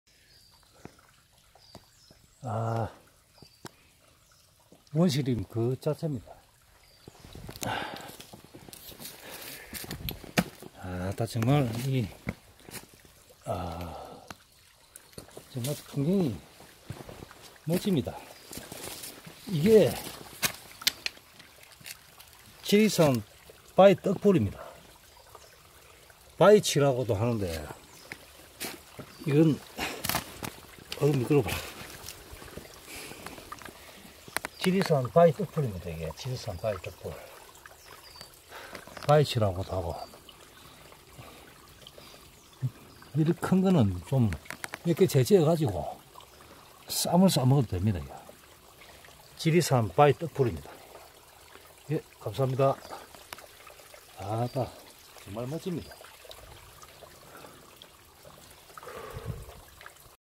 고산 지리산바위떡풀(바위취)
주로 고산지역 생태환경이 잘 보존된 곳에서만 볼수 있는 지리산 바위떡풀입니다. 바위취라고도 하며 아식한 맛과 맛은 별로 없지만 돌나물처럼 비슷한 맛이나고 인체에는 이주 이로운ㄴ 성분들을 많이 함유하고 있습니다.